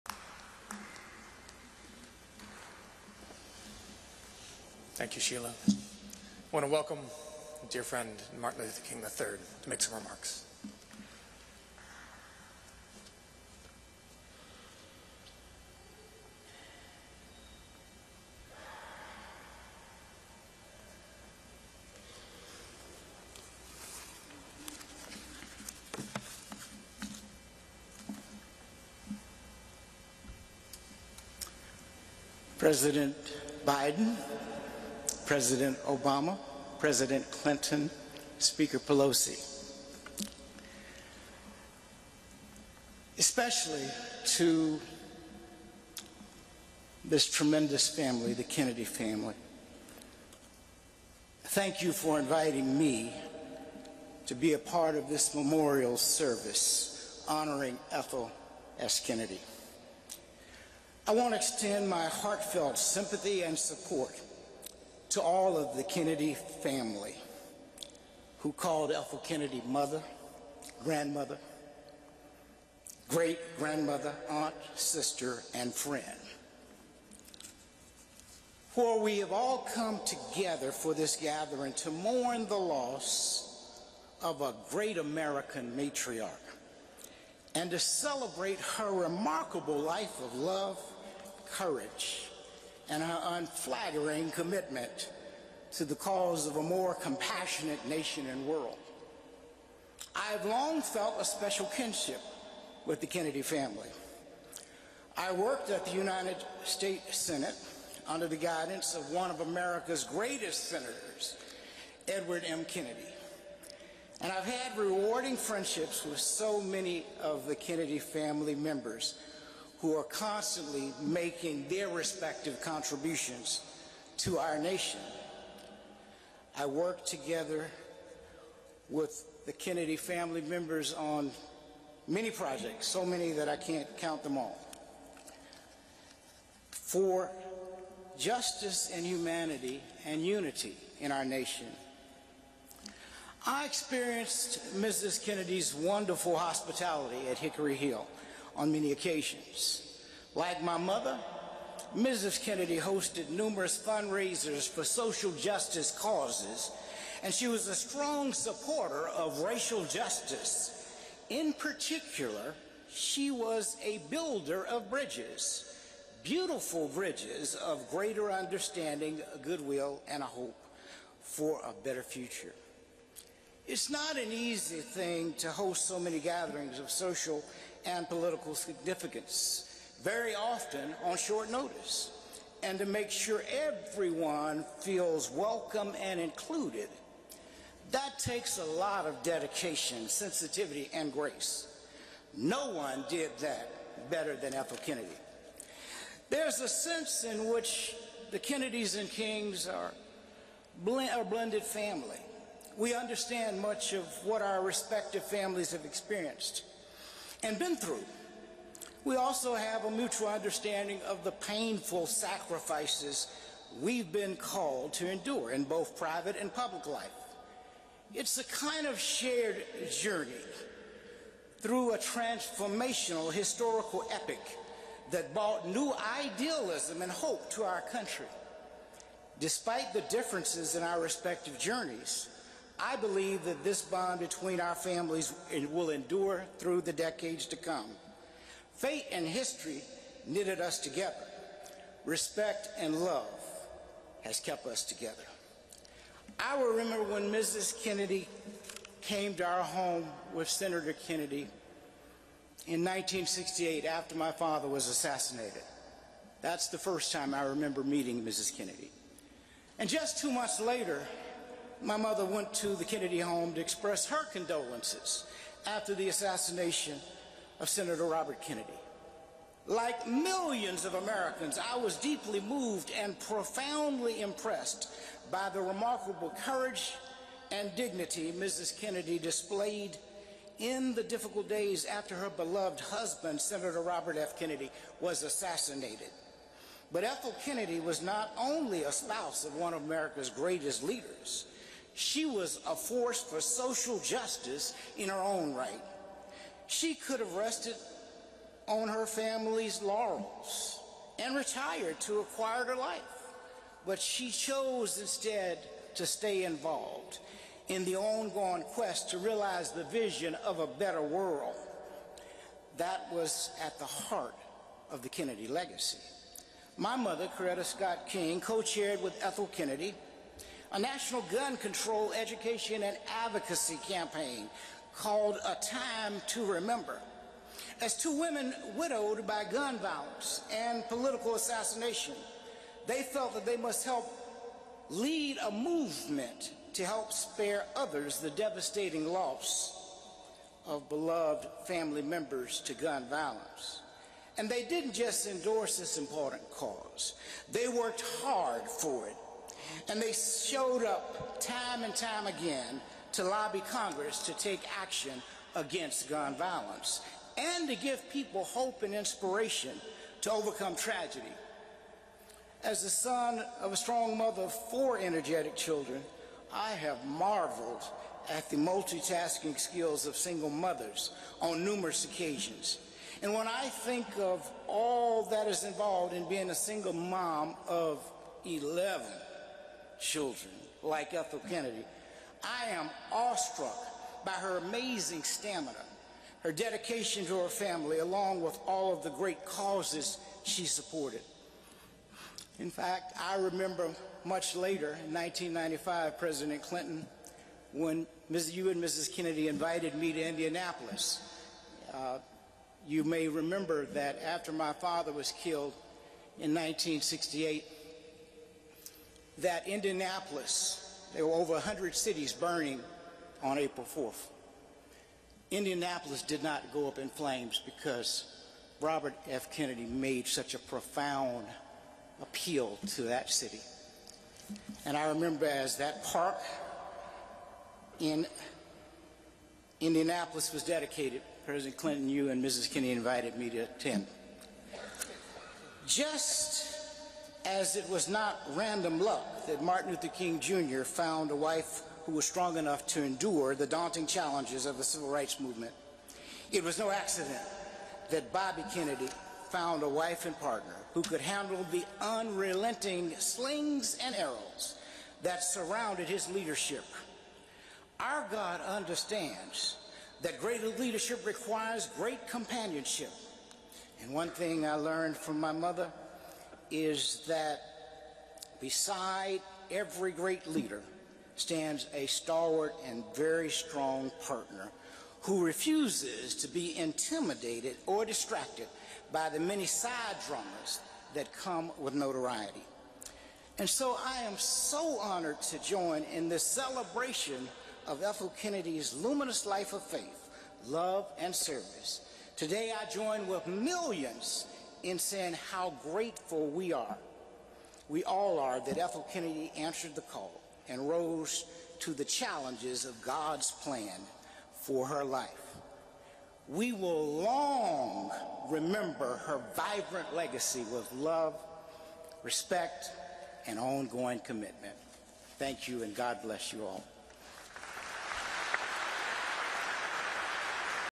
JUST IN: Martin Luther King III Remembers The Late Ethel Kennedy: 'A Great American Matriarch'
Martin Luther King III, son of Dr. Martin Luther King Jr., delivers remarks at the memorial service for Ethel Kennedy at the Cathedral of St. Matthew the Apostle in Washington, DC on Wednesday.